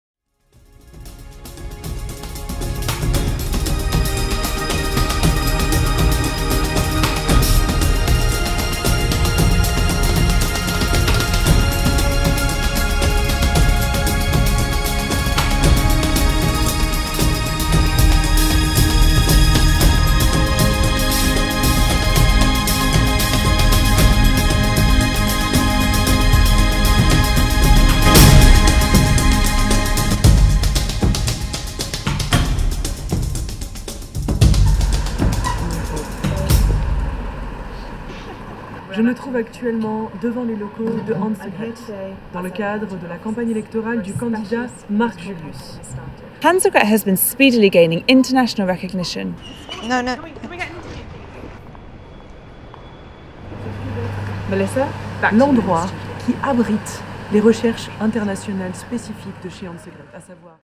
Hansegret Lifesciences - who are they?
Global company Hansegret Corporation's Lifesciences division has been gaining recent media attention.